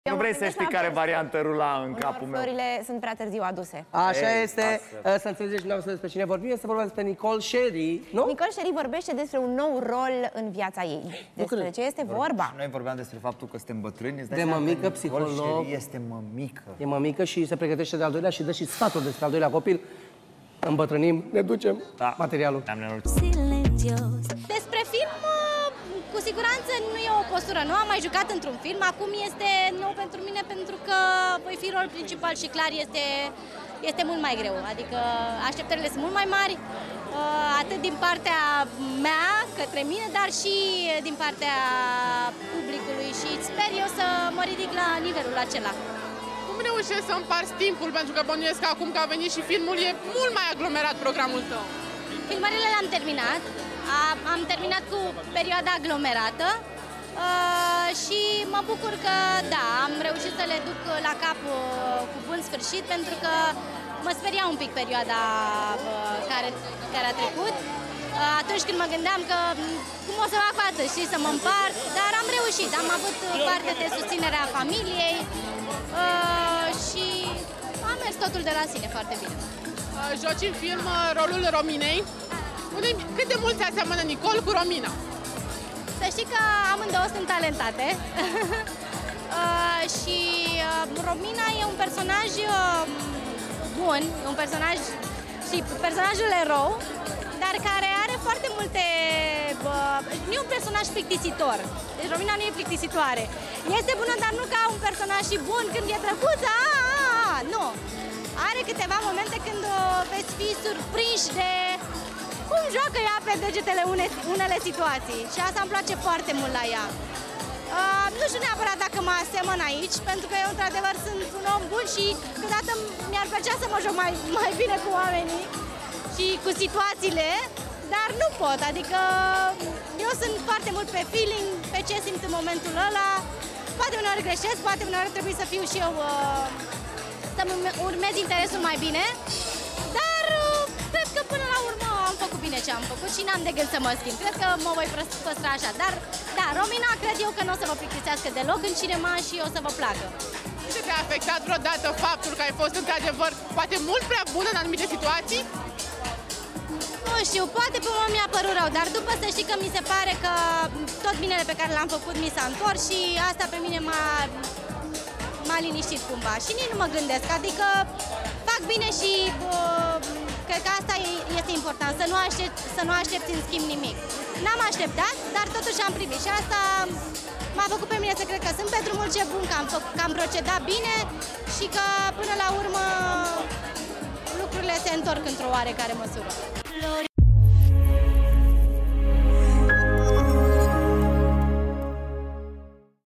Nicole Cherry se pregătește pentru cel mai mare rol de până acum!
Nicole Cherry vorbește despre un nou rol în viața ei. Iată ce a spus artista despre viitoarele ei planuri artistice.